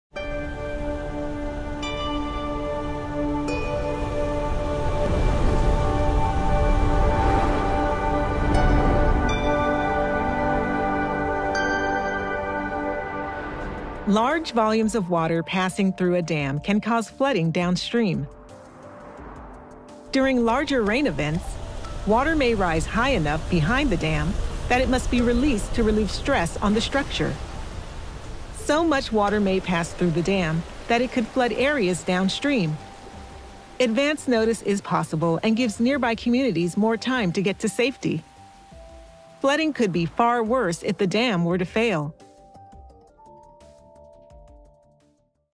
Visualizing Dams: Operational Flood Risk
The Visualizing Dams animation series explains the basics of dams including how they work and how they can break or fail. This video shows flooding that can occur from large volumes of water passing through a dam, sometimes called operational flood risk.